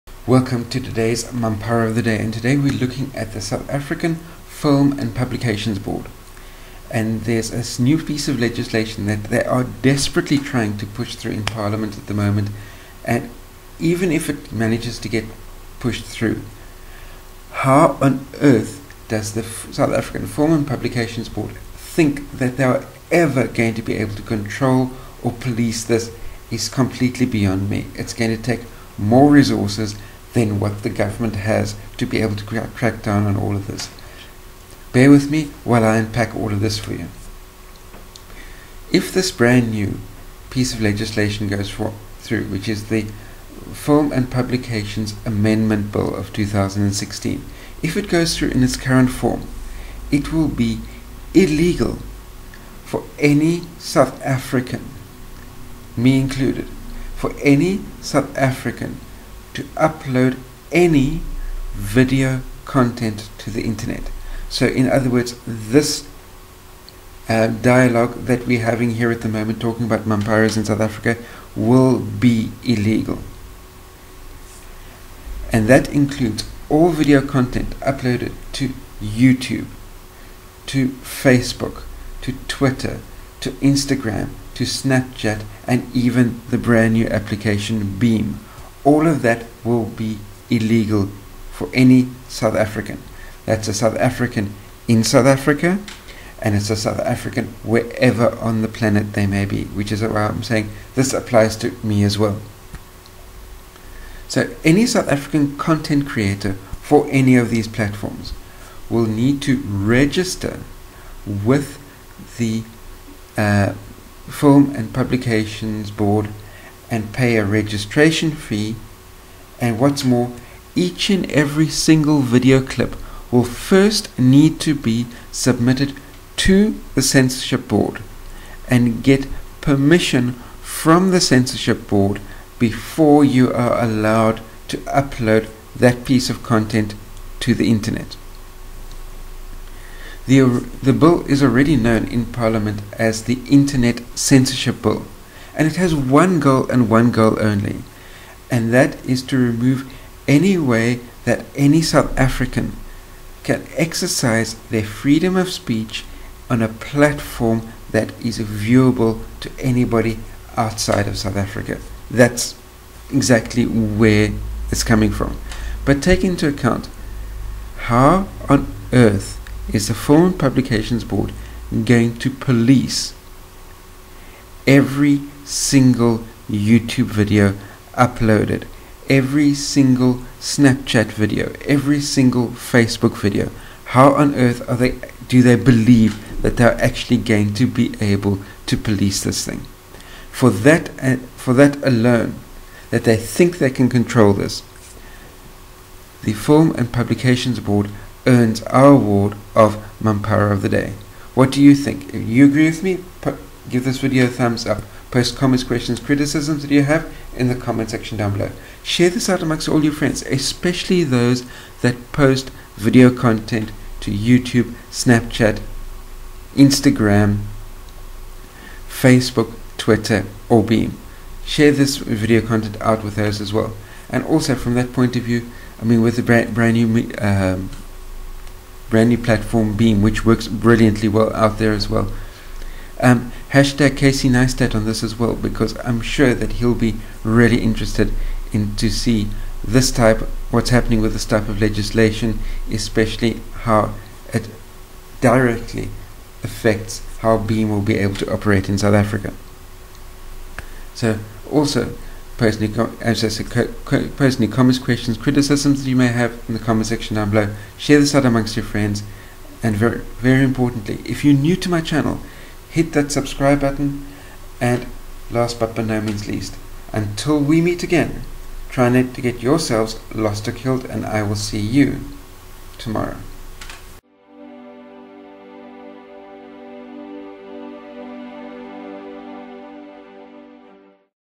Film and Publication Board of South Africa is Mampara of the Day
How do you ensure that every piece of video content posted to the internet by South Africans carries the sweet perfume of rose petals?

You create a law where every single South African, no matter where in the world, needs to register and pay for the privilege of being a film distributor.

Then you add another layer of rose petals where each piece of video content must be approved by the censor board before it can be uploaded.

For example, in YouTube, each uploaded video will need 100 000 views to recover the censorship board application fee for that single video.

In addition, because the government will now know about you, the censorship board will be allowed to get take down notices against your videos and channel.